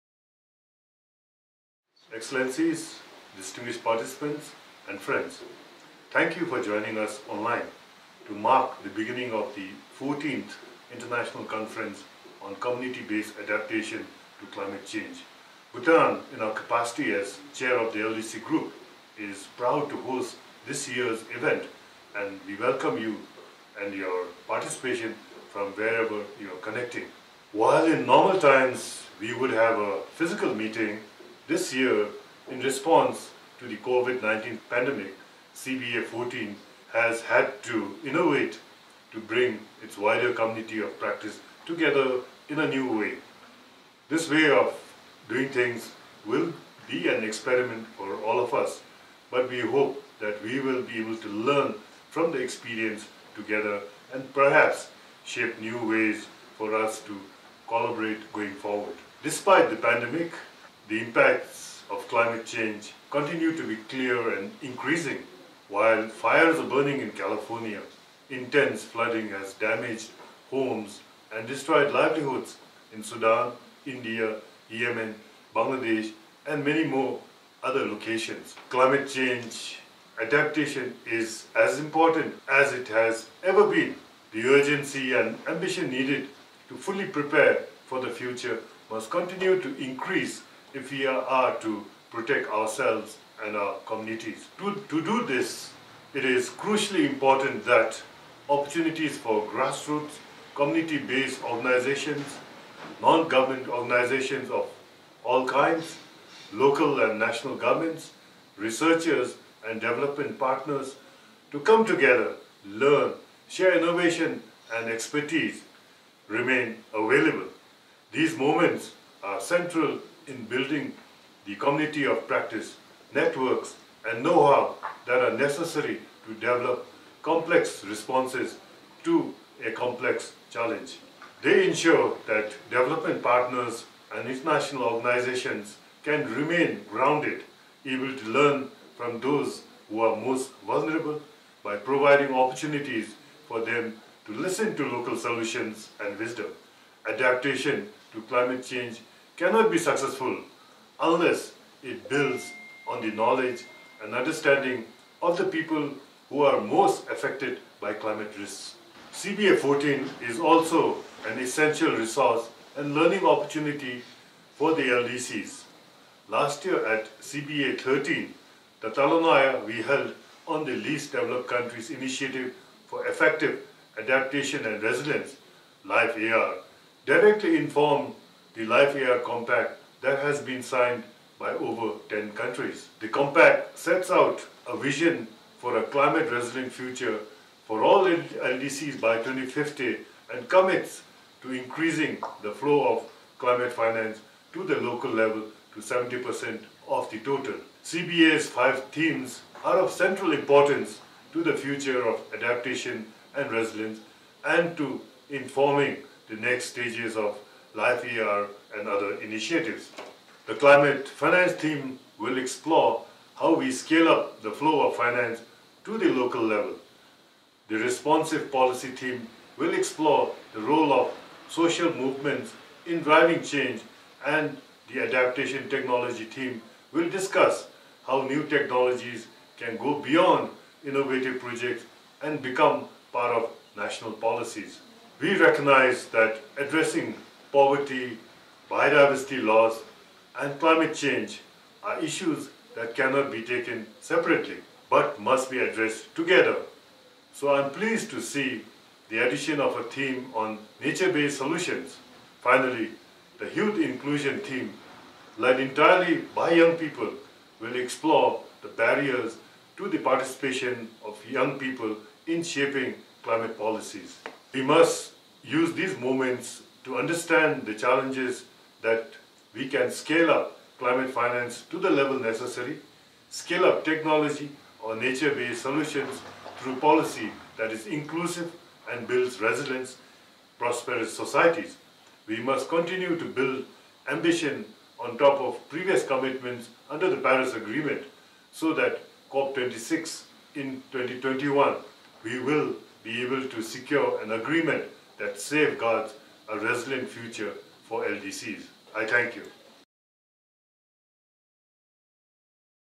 CBA14: Welcome address by the LDC Group chair
Sonam P Wangdi, chair of the Least Developed Countries Group of the UNFCCC makes opening remarks at the 14th International Conference on Community-Based Adaptation to Climate Change (CBA14), which is taking place online from 21-25 September 2020.

CBA14 is the leading practitioner-focused forum on climate adaptation, delivering dialogue and evidence to inform policy and action – from the local to the global scale. It will bring together practitioners, grassroots representatives, policymakers and donors for interactive discussions, workshops and skillshares.

Wangdi, from Bhutan, reflected on COVID-19 and how, despite the pandemic, the impacts of climate change continue to be clear and increasing and that "climate change adaptation is as important as it has ever been".

He said: "The urgency and ambition needed to fully prepare for the future must continue to increase if we are to protect ourselves and our communities. To do this, it is crucially important that opportunities for grassroots, community-based organisations, non-government organisations of all kinds, local and national governments, researchers and development partners to come together, learn, share innovation and expertise remain available. These moments are central in building the community of practice, networks and knowhow that are necessary to develop complex responses to a complex challenge."